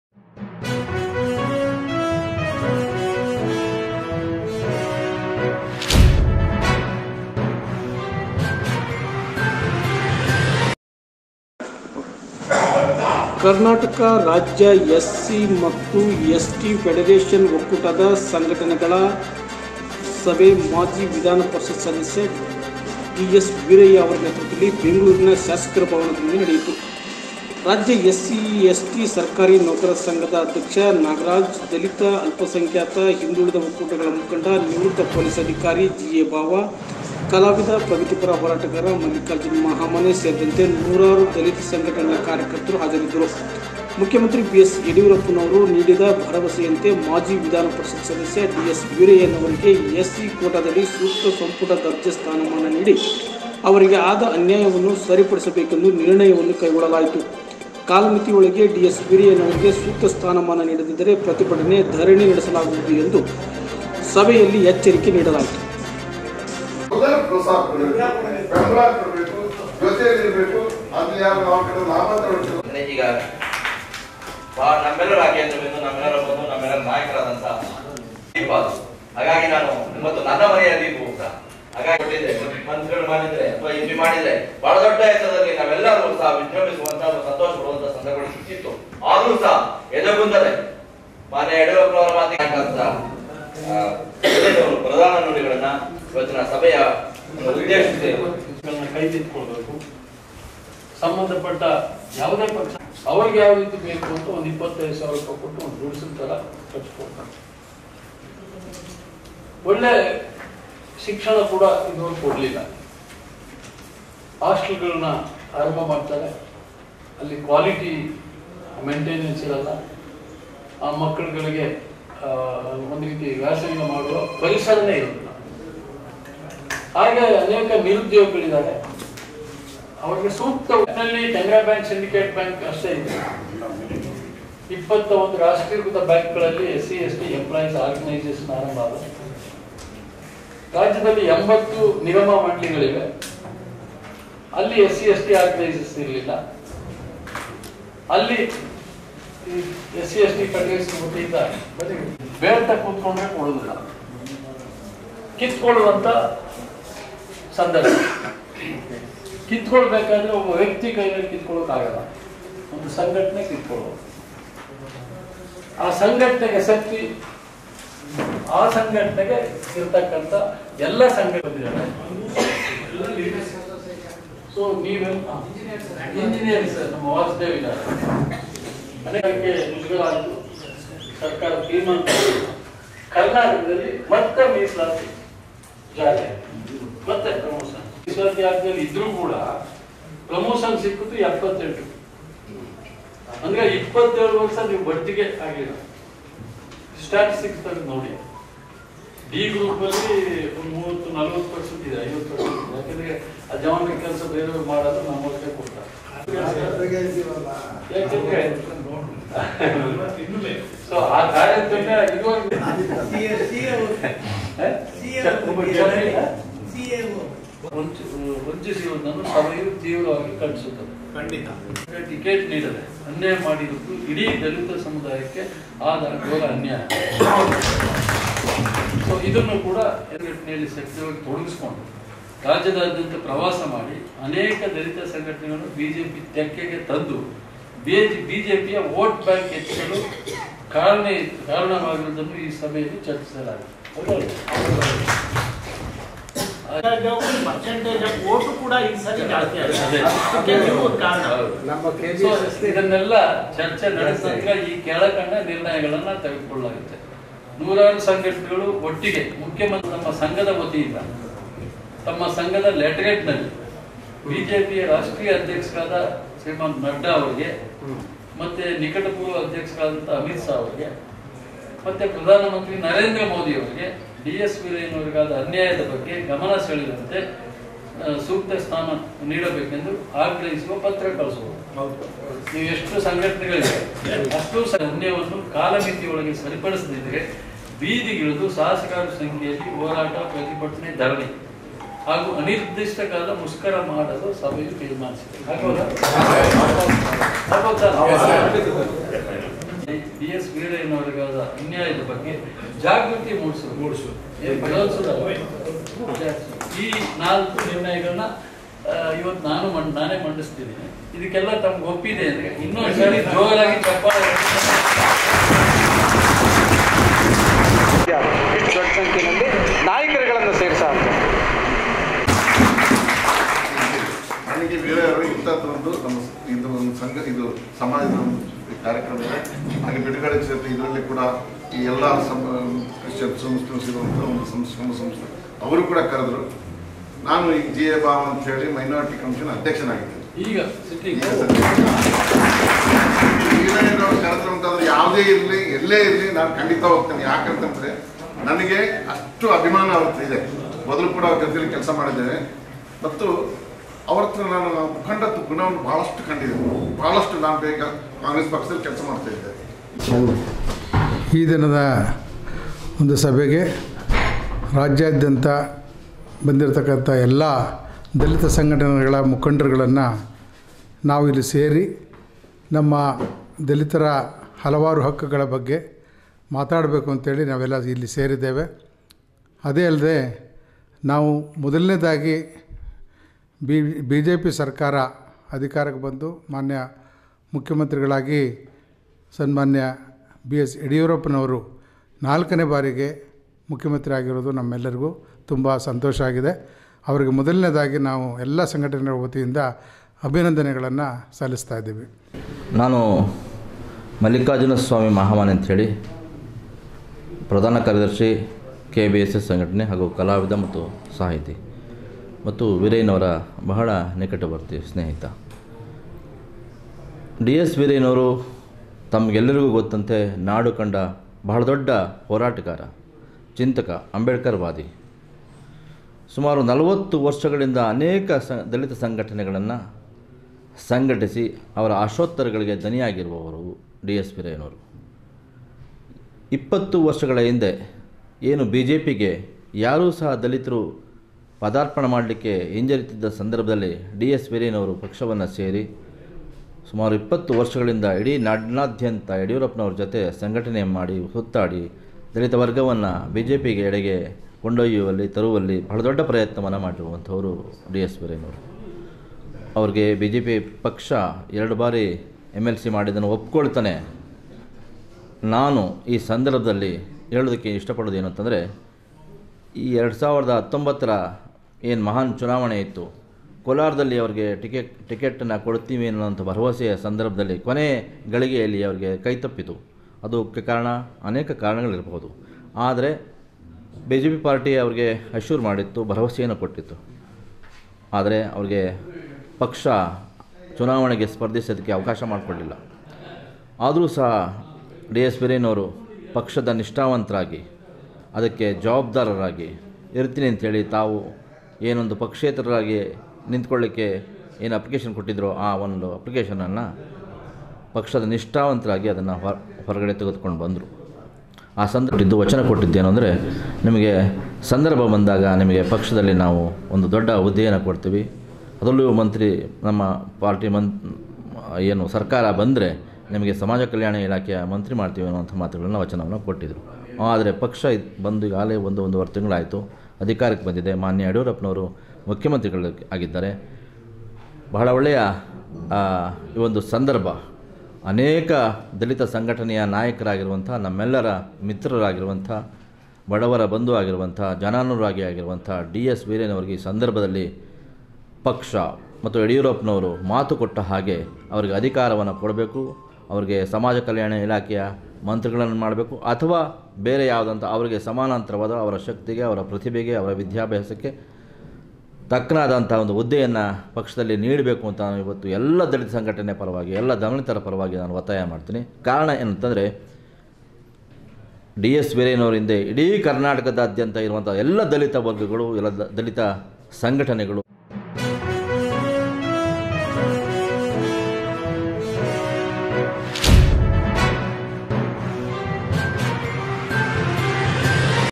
ರಾಜ್ಯ ಎಸ್ ಸಿ ಎಸ್ಟಿ ಪರಡರೇಷನ್ ಸಂಘಟನೆಗಳ ಒಕ್ಕೂಟ
ರಾಜ್ಯ ಎಸ್.ಸಿ.ಎಸ್ಟಿ ಪರಡರೇಷನ್ ಸಂಘಟನೆಗಳ ಒಕ್ಕೂಟ ಸಭೆ ಮಾಜಿ ವಿಧಾನ ಪರಿಷತ್ ಸದಸ್ಯ ಡಿ.ಎಸ್.ವೀರಯ್ಯ ನೇತೃತ್ವದಲ್ಲಿ ಬೆಂಗಳೂರಿನಲ್ಲಿ ನಡೆಯಿತು. ಡಿ.ಎಸ್.ವೀರಯ್ಯ ನವರಿಗೆ ಸಚಿವ ಸಂಪುಟದಲ್ಲಿ ಸೂಕ್ತ ಸ್ಥಾನಮಾನ ನೀಡುವಂತೆ ಮುಖ್ಯಮಂತ್ರಿ ಯಡಿಯೂರಪ್ಪ , ರಾಷ್ಟ್ರೀಯ ಅಧ್ಯಕ್ಷ ಅಮಿತ್ ಶಾ, ಪ್ರಧಾನ ಮಂತ್ರಿ ನರೇಂದ್ರ ಮೋದಿಯವರಿಗೆ ಒತ್ತಡ ಹೇರುವ ಬಗ್ಗೆ ನಿರ್ಣಯ ಕೈಗೊಳ್ಳಲಾಯಿತು.